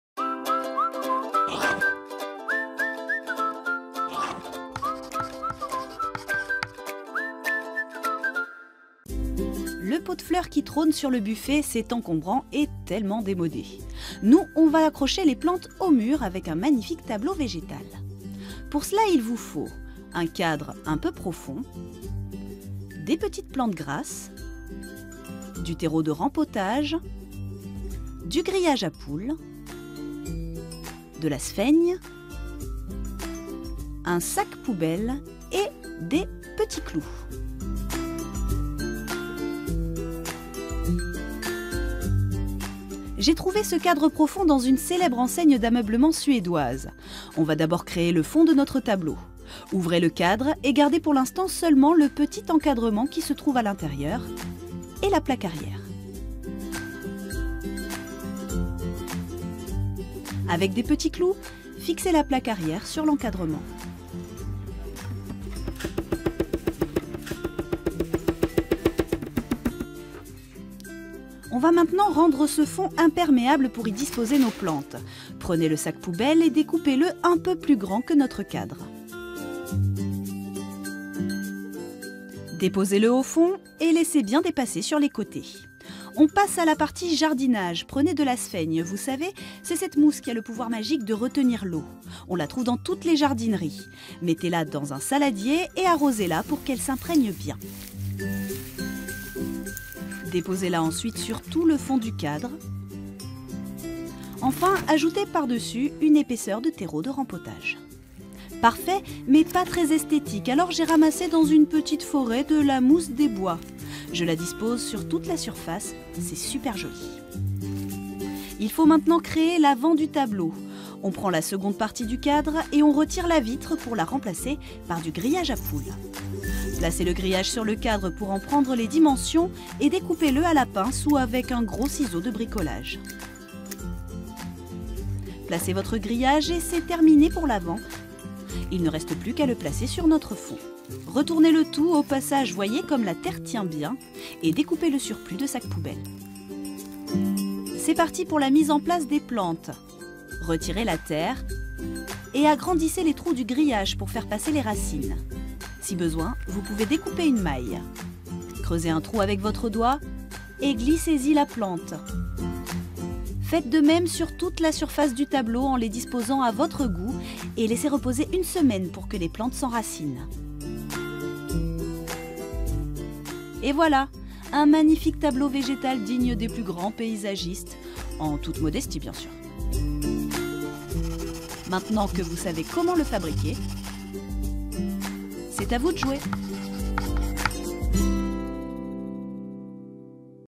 Comment réaliser un « tableau végétal » pour décorer votre intérieur ?
Amélie vous propose un Tuto Brico pour créer facilement des objets de décoration tendance. Pour changer du pot de fleurs qui trône sur le buffet du salon, nous vous proposons d'accrocher les plantes au mur, grâce à un tableau végétal.

Vous aurez besoin :

- D'un cadre un peu profond
- Des petite plantes grasses
- Du terreau de rempotage
- Du grillage à poules
- De la sphaigne
- Un sac poubelle
- Quelques petits clous

Le principe est simple : dans un cadre que vous achèterez dans le commerce, vous installerez du terreau et de la sphaigne. Une fois le tout fermé par du grillage à poules, vous glisserez quelques plantes grasses. Après une semaine de repos pour que les plantes s'enracinent, vous pourrez accrocher votre tableau au mur et épater vos invités ! Do it yourself ?